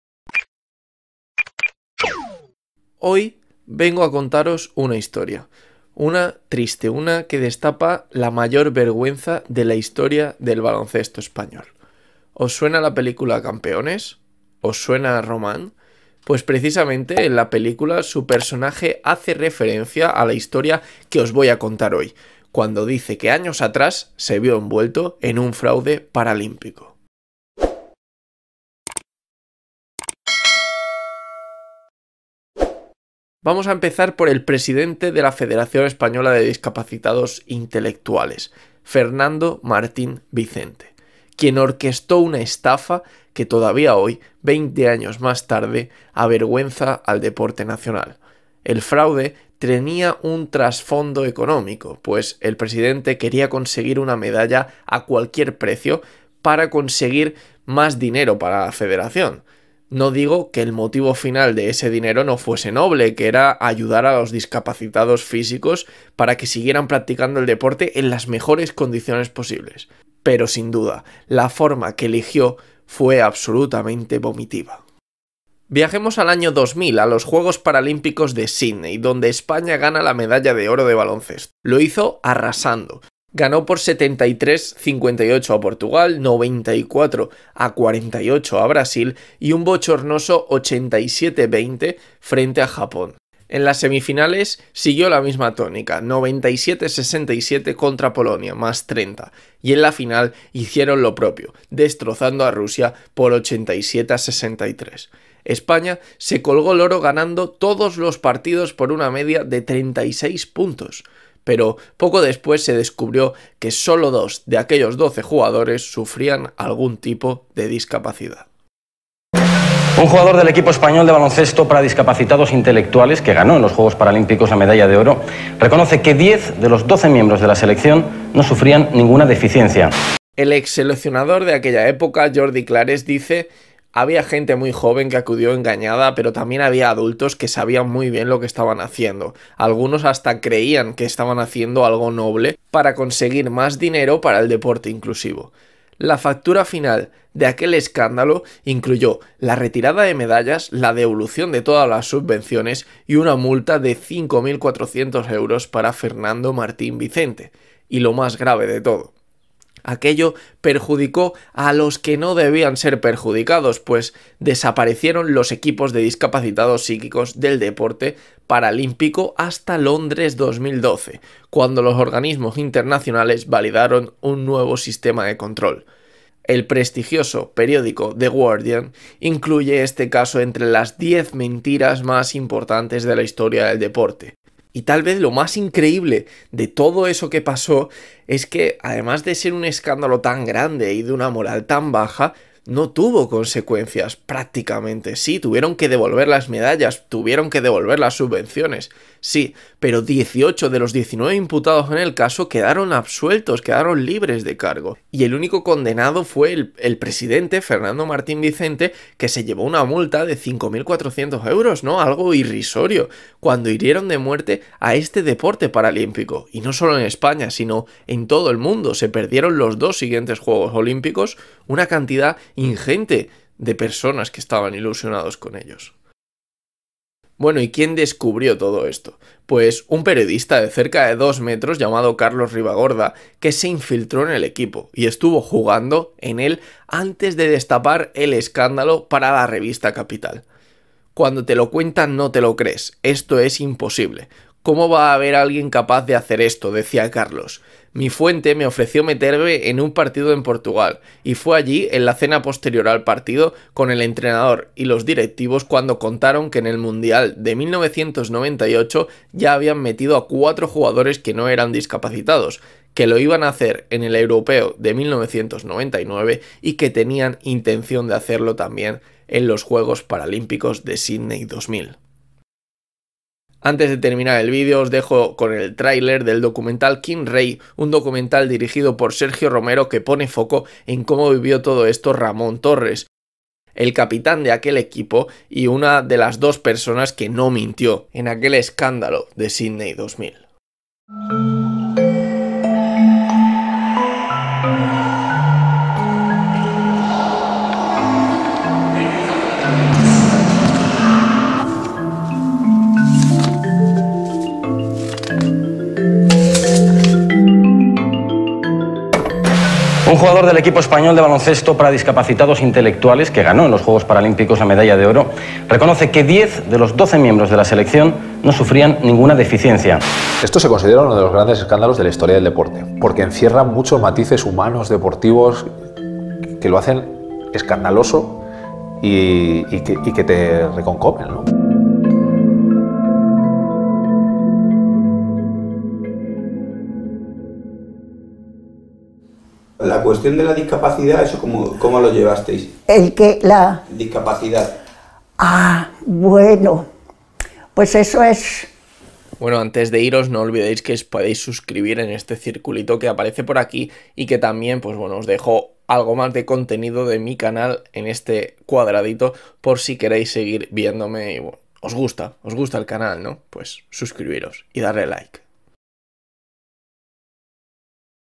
FRAUDE BALONCESTO SIDNEY 2000 😱😱😱
Hoy os voy a contar una historia, una triste, una que destapa la mayor vergüenza de la historia del baloncesto español.

¿Os suena la película campeones? ¿Os suena Román? Pues precisamente en la película su personaje hace referencia a la historia que os voy a contar hoy cuando dice que años atrás se vio envuelto en un fraude paralímpico.

El presidente de la Federación Española de Discapacitados Intelectuales, Fernando Martín Vicente, orquesto una estafa que todavía hoy, 20 años más tarde, avergüenza al deporte nacional. El fraude tenía un trasfondo económico, pues el presidente quería conseguir una medalla a cualquier precio porque conseguir una medalla significaba más dinero para su Federación. No digo que su motivo final no fuese noble, solo quería que los discapacitados físicos pudieran seguir practicando deporte en las mejores condiciones posibles, pero sin duda, la forma que eligió fue absolutamente vomitiva.

¿Y quién descubrió todo esto? Pues un periodista de cerca de 2 metros llamado Carlos Ribagorda, que se infiltró en el equipo y estuvo jugando en el antes de destapar el escándalo para la revista CAPITAL.